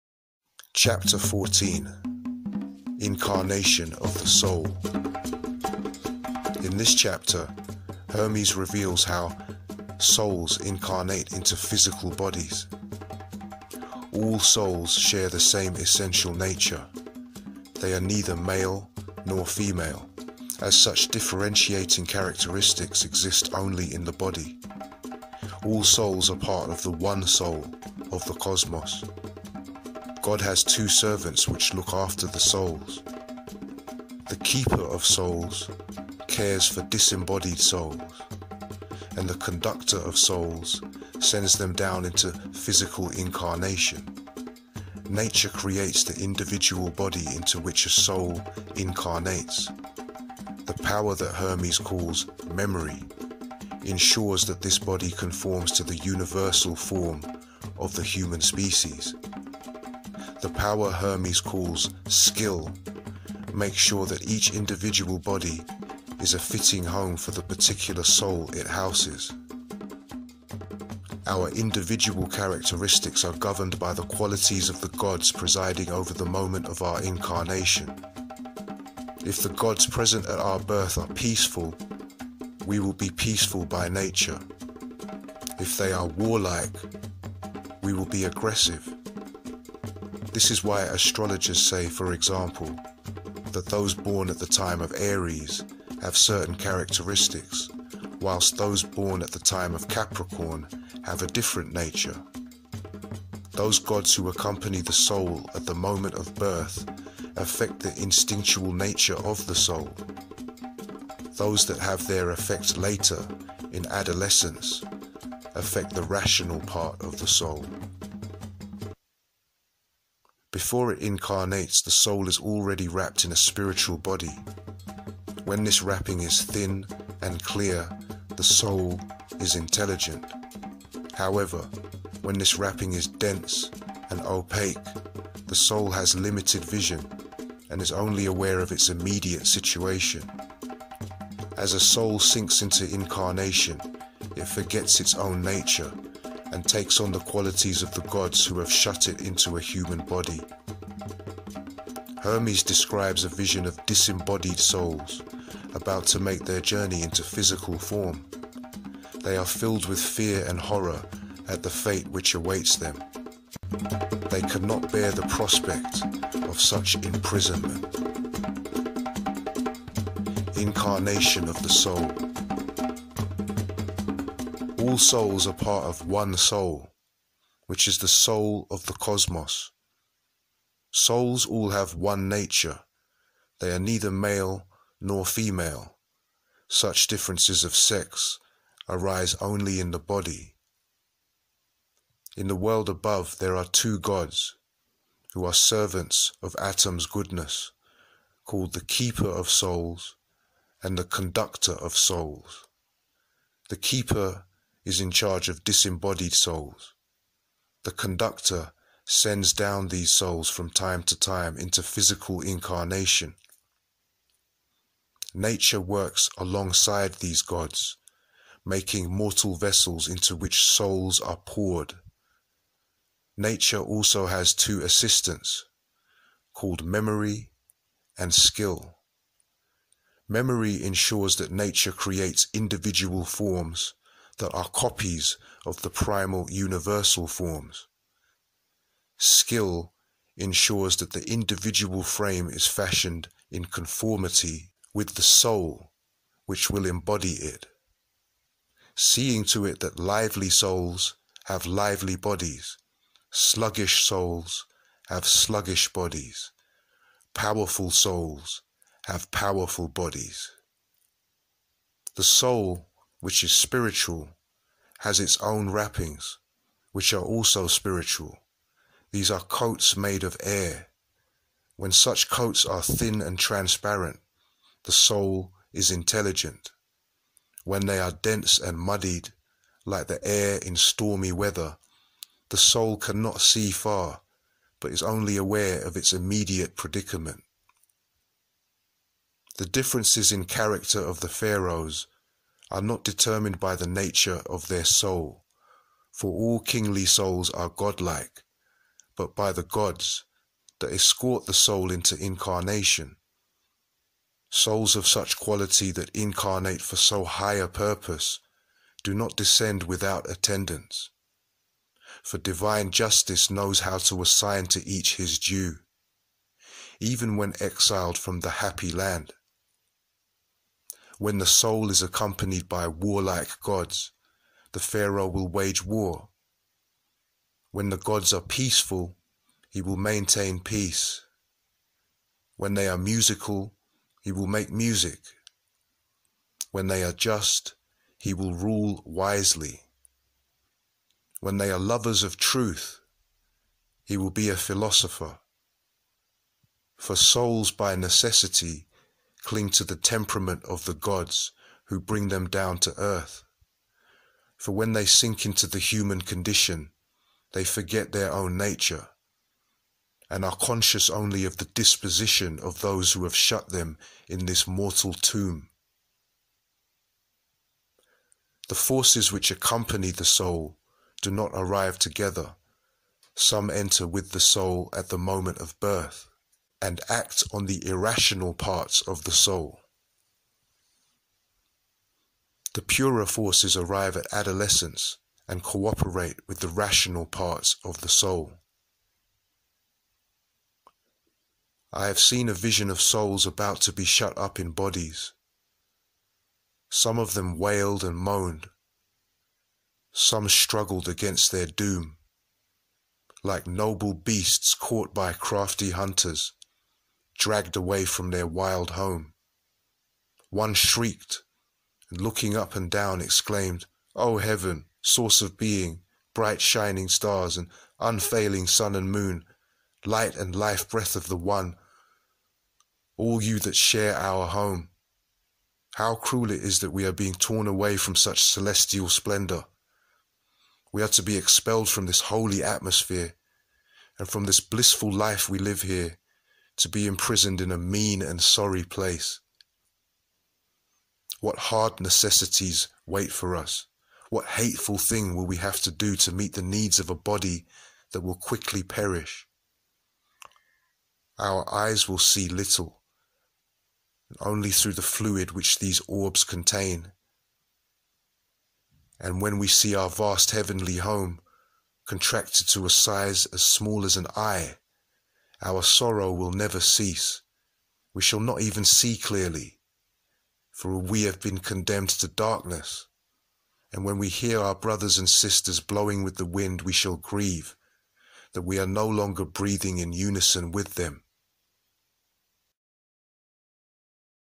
INCARNATION OF THE SOUL
INCARNATION OF THE SOUL - Chapter 14 - The Hermetica AUDIOBOOK || Timothy Freke & Peter Gandy